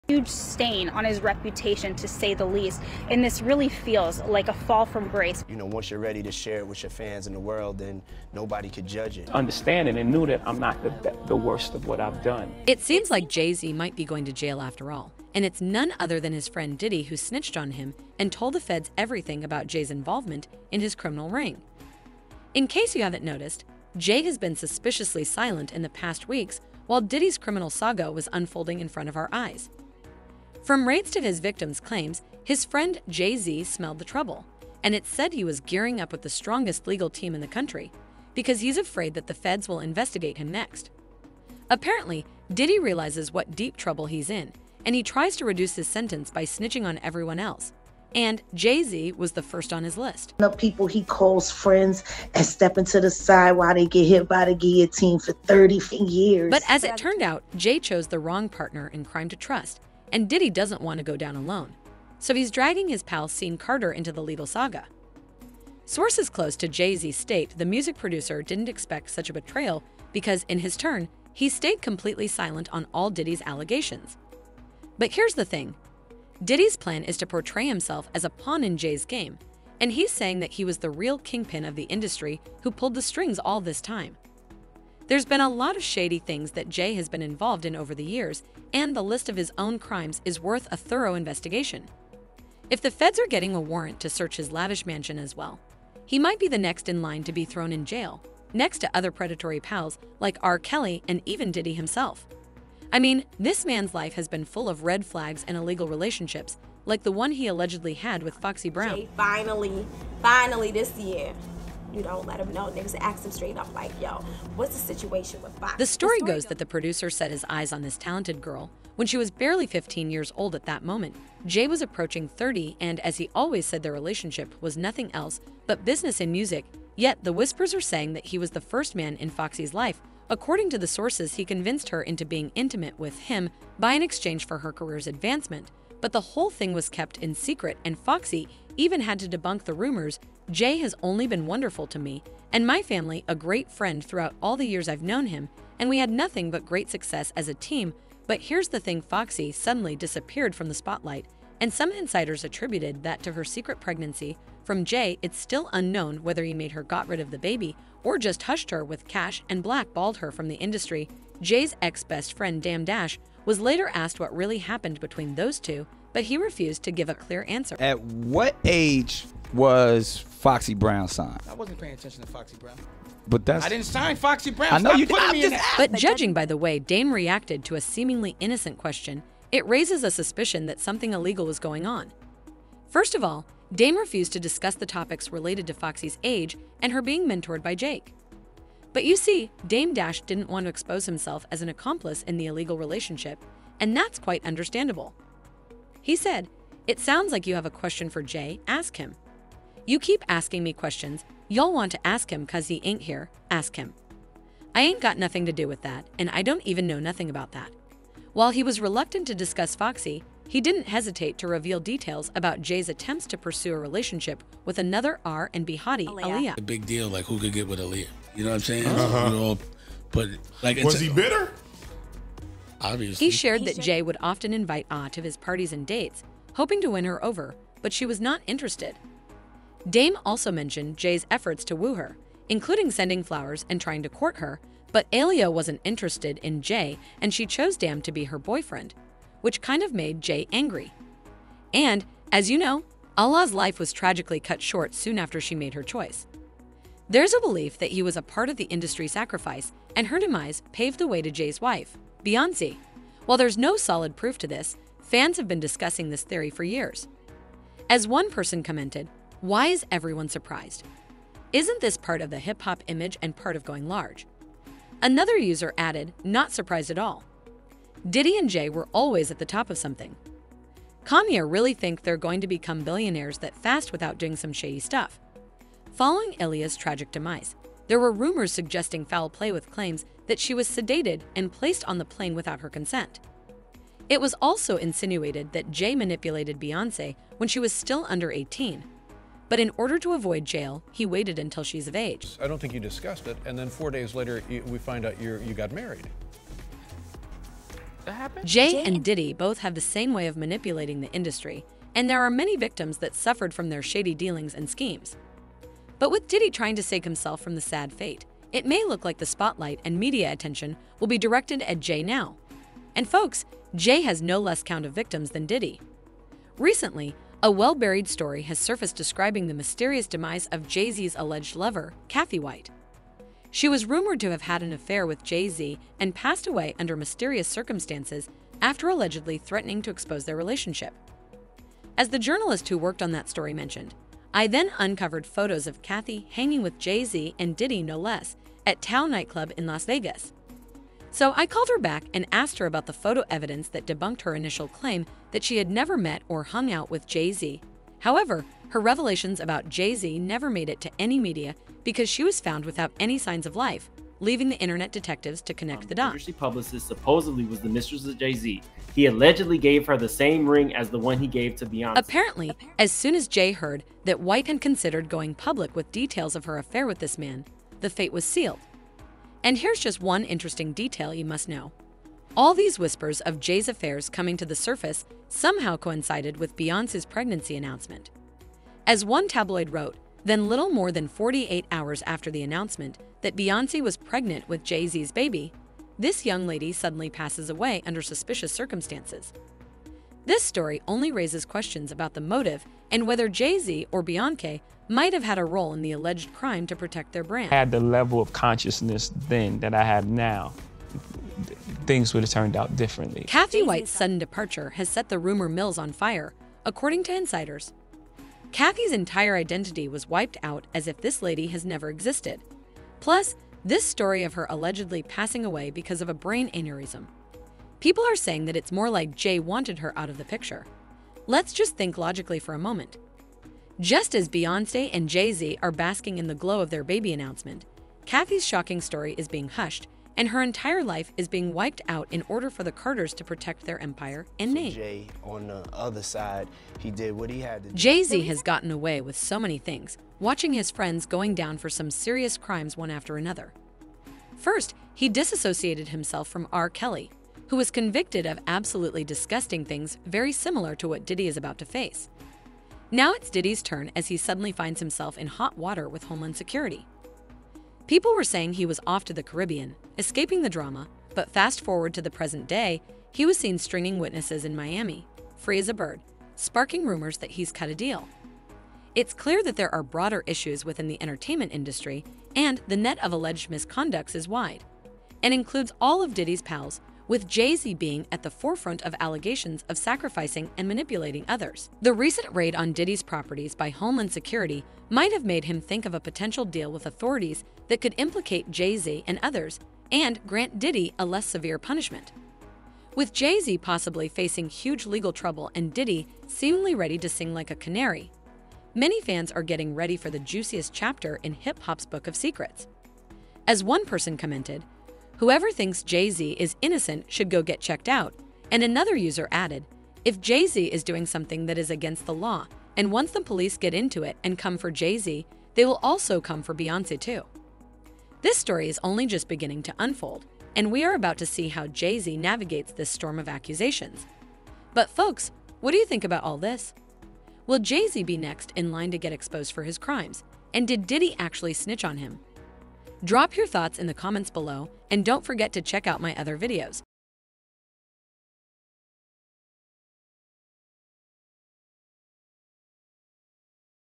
7 Minutes Ago: Diddy Drops Bombshell on Jay Z's Criminal Enterprise
It appears Jay-Z may be facing serious legal trouble, with his close friend Diddy reportedly providing detailed information to federal authorities about Jay's alleged involvement in criminal activities.

In this video, we delve into the intricacies of Diddy's involvement, the reported raid on his residence, and the potential implications for both him and Jay-Z. From discussions about Diddy's connections to Sean Combs, Puff Daddy, and P. Diddy to the latest updates on the raid and its aftermath, we cover a wide range of related topics, including reactions from the music industry and the public.

Additionally, we explore other angles such as Diddy's encounters at Miami airport, connections with 50 Cent and Jay-Z, and speculation surrounding his potential evasion tactics. Join us as we unpack the unfolding drama surrounding Diddy's reported involvement and Jay-Z's purported panic in the wake of these revelations.